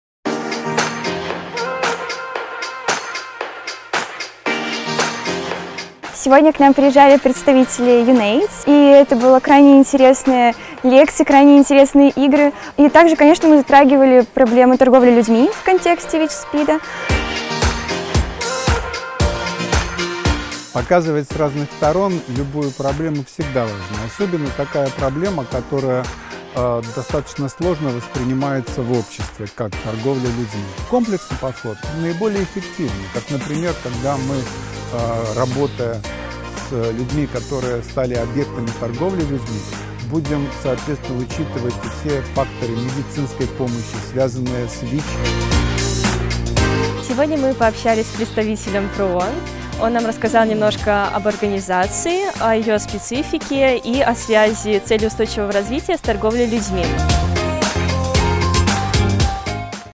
DAY 3 - "LEARN.ACT.SHARE" Youth summer camp diary 2018
On 16th of July the 2nd “Lear.Act.Share” youth summer camp kicked off near Minsk, in the very center of Belarus, uniting 18 active young people from different regions in their desire to LEARN about human trafficking problem and then ACT and SHARE the knowledge in their communities.
6-day summer camp offers to the participants interactive and informative workshops, trainings and master-classes to educate 18 selected participants on human trafficking and related issues and on how to tell about human trafficking and related issues and what tools to use to make a difference. The young people will end up with their own thematic preventive initiatives and mini-projects that they will implement throughout their year with expertise support of IOM and all involved partners from different UN agencies, government, NGOs and private companies. Moreover, they will join last year summer camp alumni establishing a network of young agents in the regions to contribute to the fight with modern slavery.
The youth summer camp is tailored to the World Day Against Trafficking in Persons, and is the part of the IOM X Belarus campaign launched together with the Ministry of the Interior of the Republic of Belarus with the support of USAID.

The initiative is organized with the support of the Ministries of the Interior and Foreign Affairs, UN Agencies, the British Embassy in MInsk,  USAID, the Norwegian MFA, crowdfunding platform Talaka, private companies - Logoton, LigaturaPrint, Nota Kitchen Media Production.